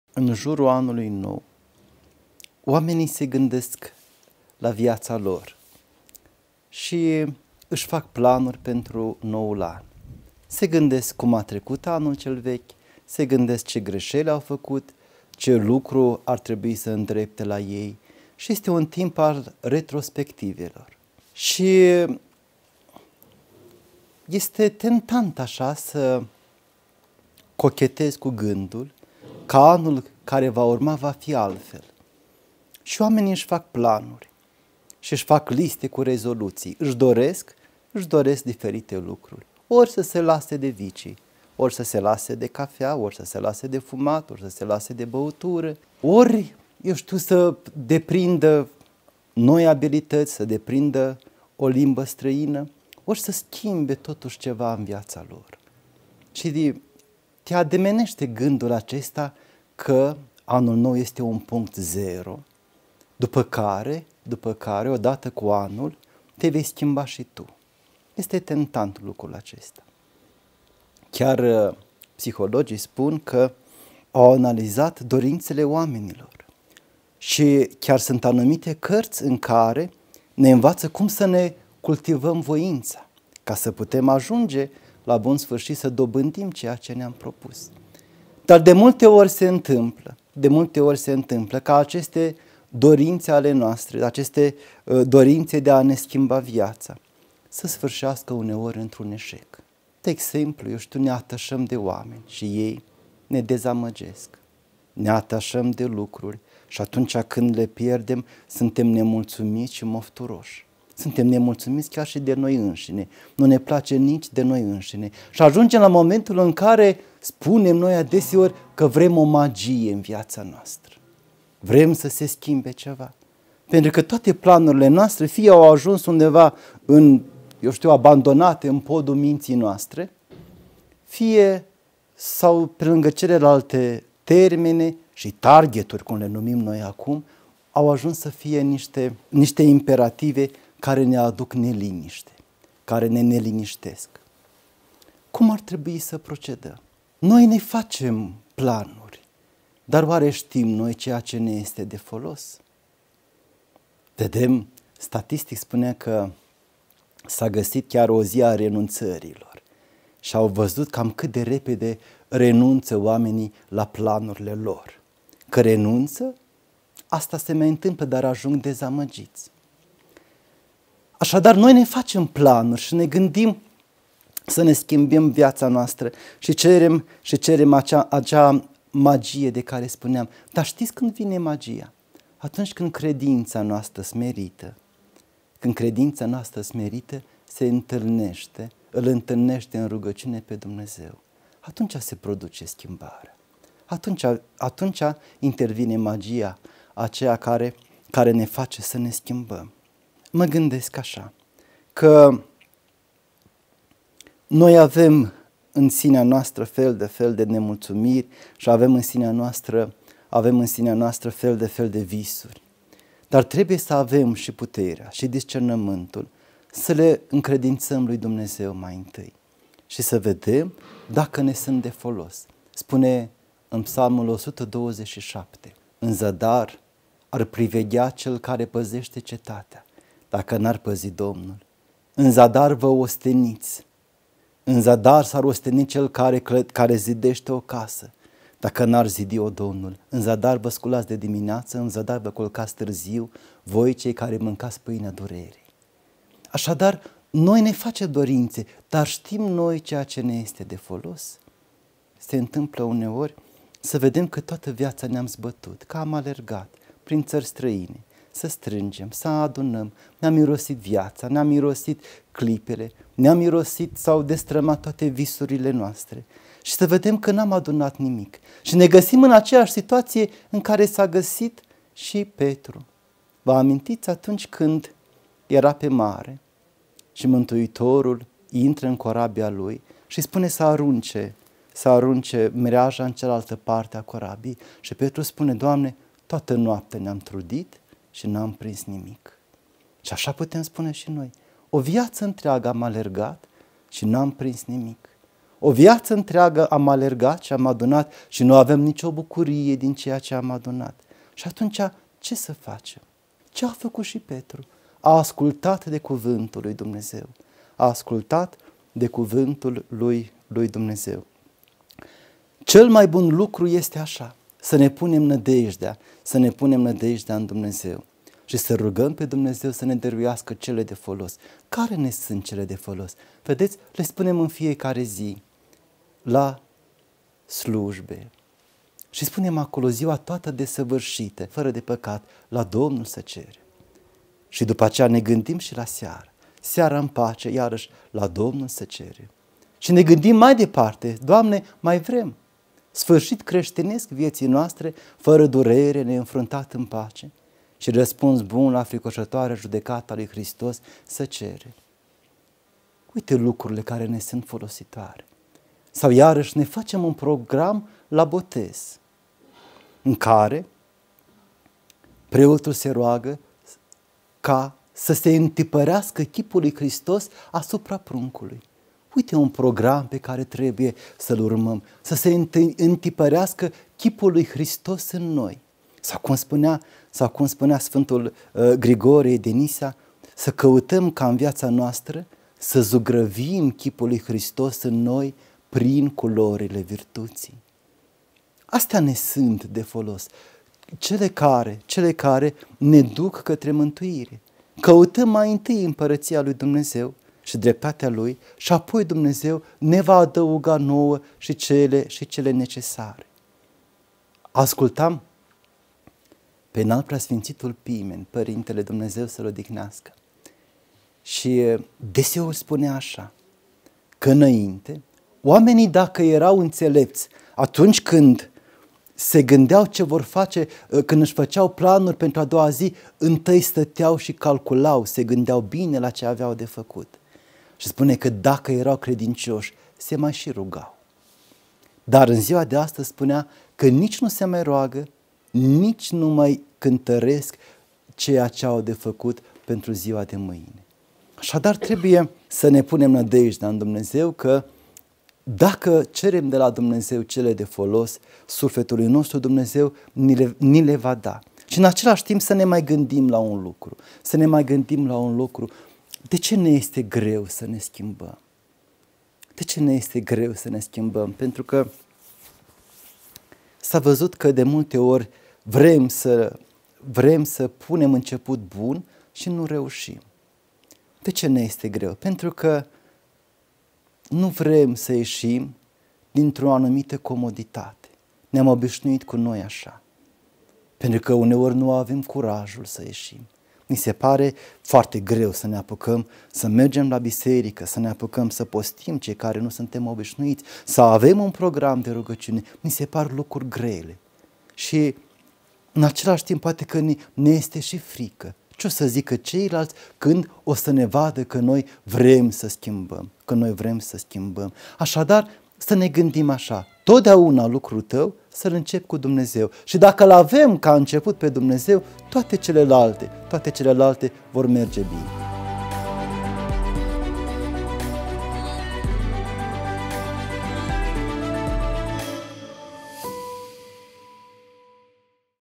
Să facem planuri la început de an?
„Să facem planuri la început de an?” - Părintele Galaction Dominte, starețul Mănăstirii Botoș, ne vorbește despre planurile pe care noi, oamenii, le facem și despre planurile lui Dumnezeu.